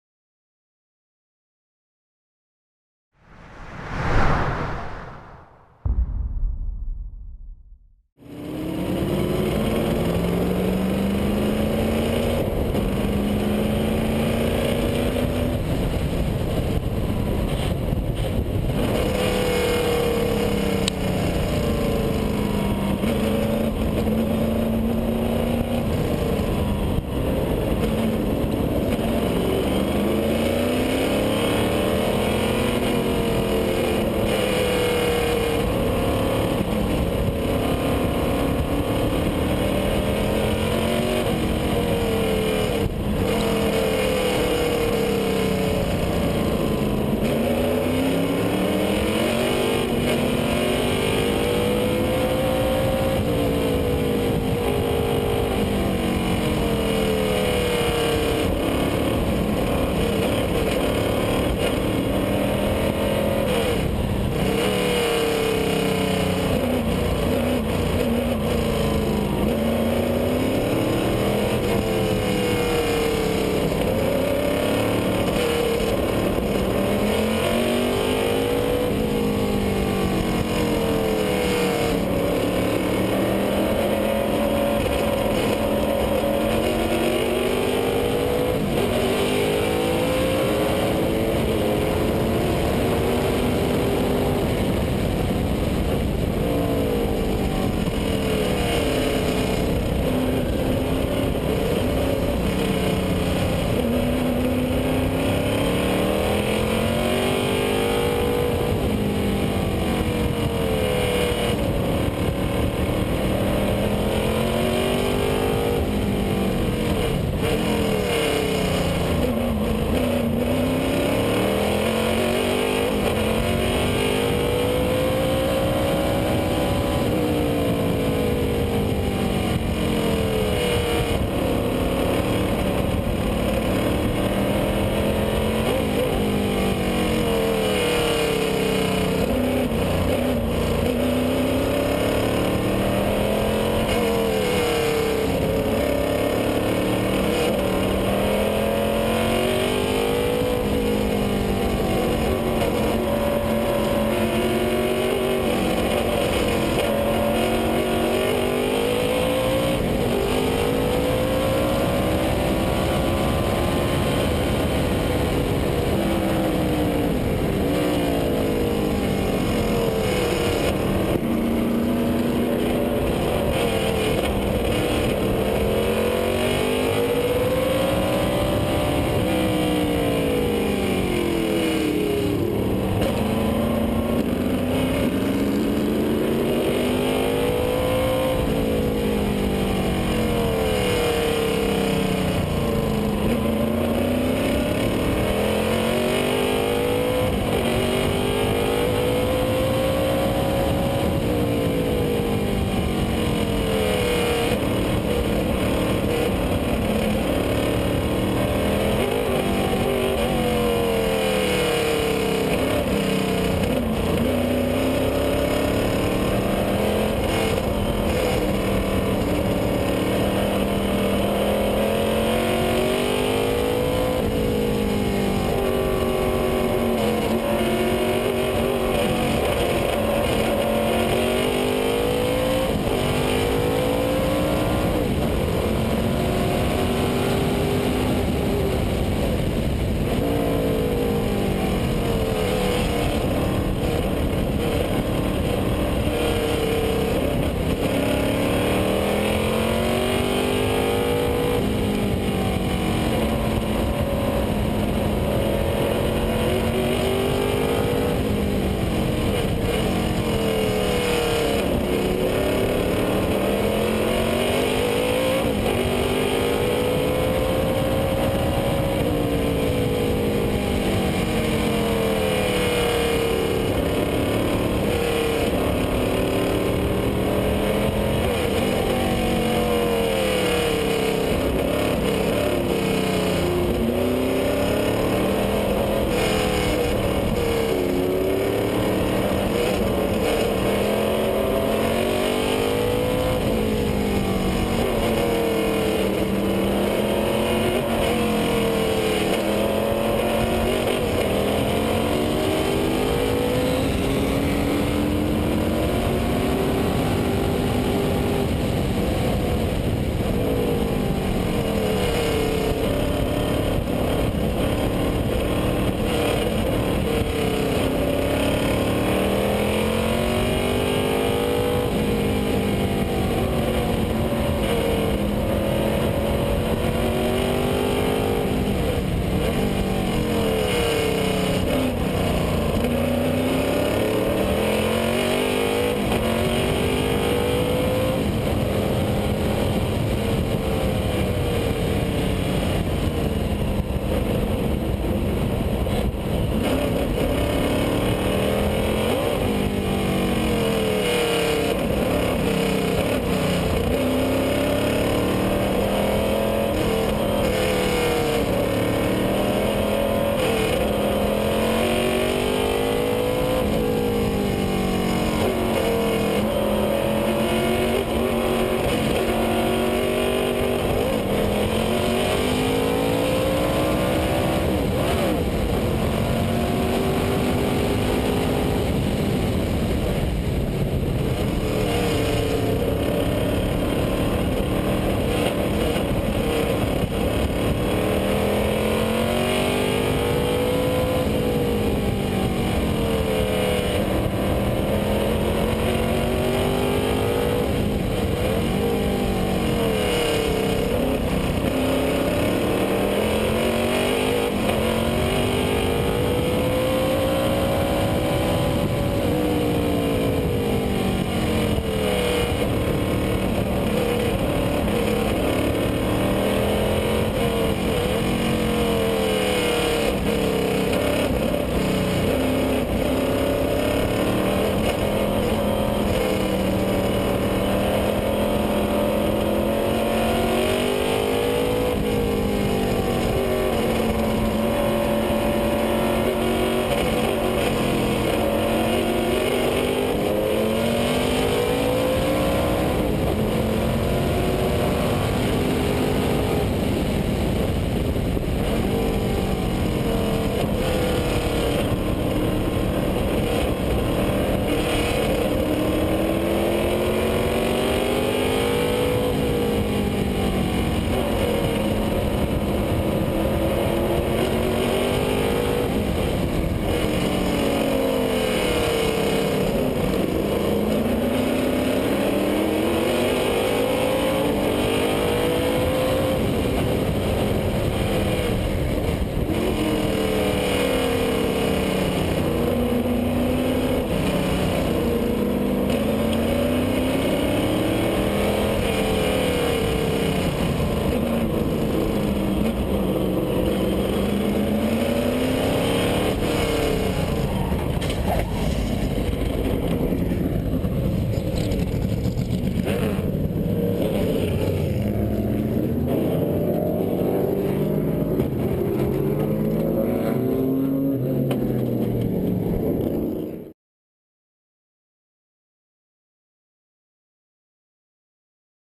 Chuck McConnell Lola Frisbee
Ride with Chuck, as he starts from the back-of-the-pack at the Coronado Speed Week.  Watch a thrilling display of driver skill and daring, as he charges through a field-of-dream Ferrari's, Lola's, and Vintage IMSA rides, on a very bumpy airstrip, only to be shortchanged before the finish!